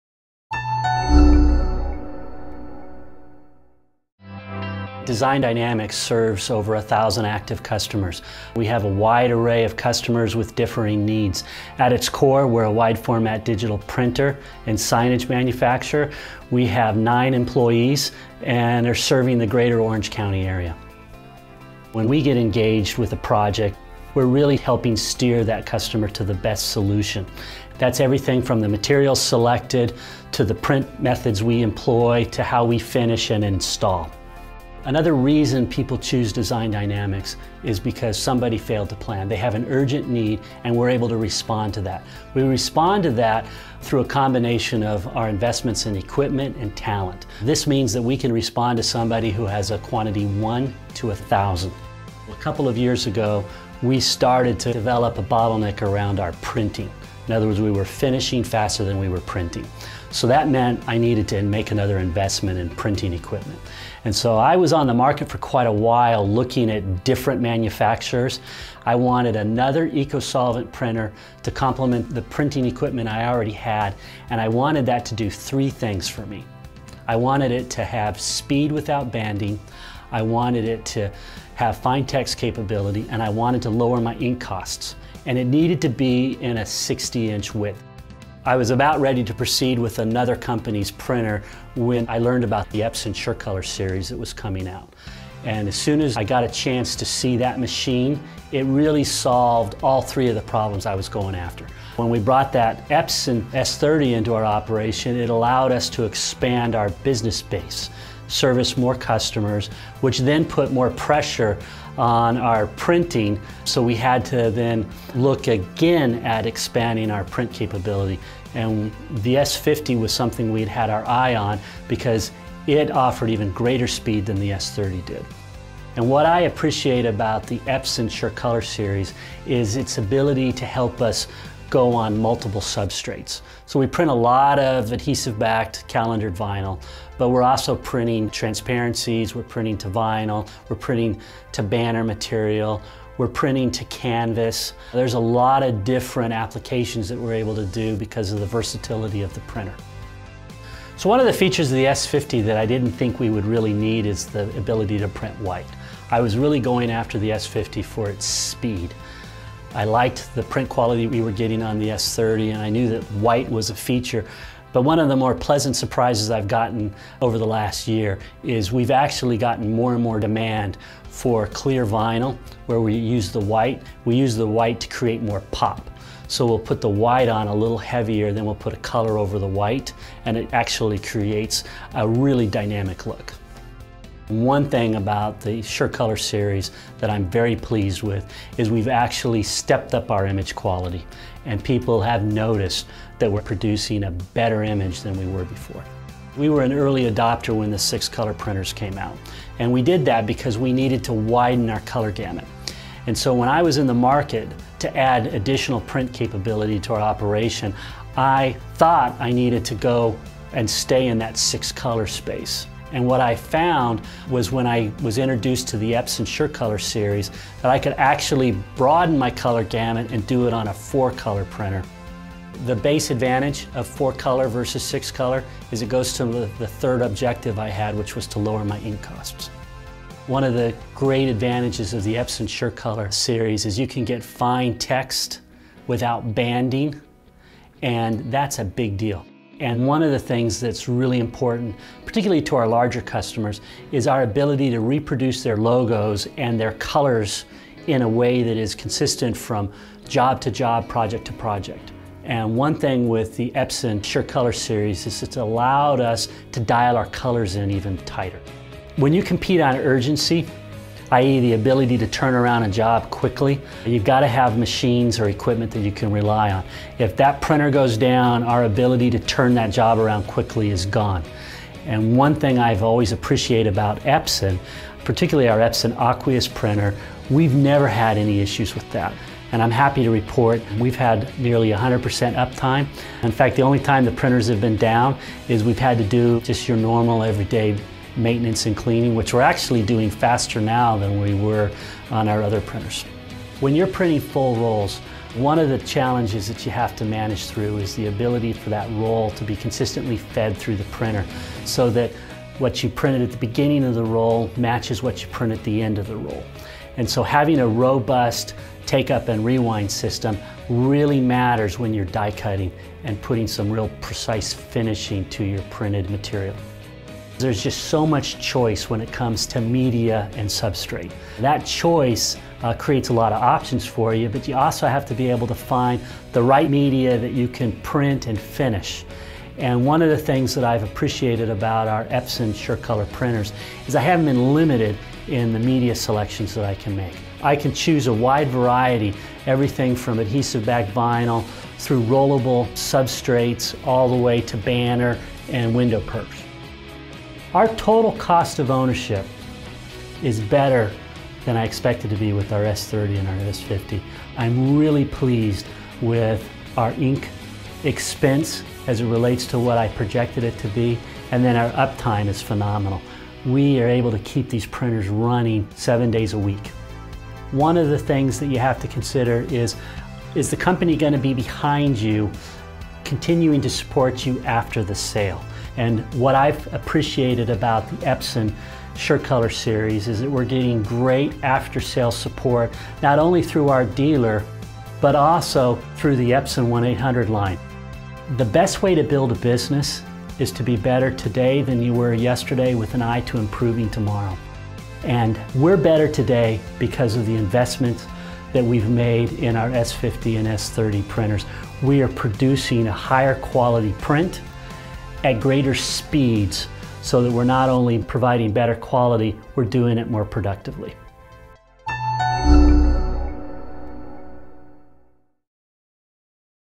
Epson SureColor S50675
Learn about the Epson S50675, a dual-CMYK printer with optional white ink, and how it has helped Design Dynamics produce prints at high speed, without banding, and has allowed them to print to a variety of substrates to meet customer needs.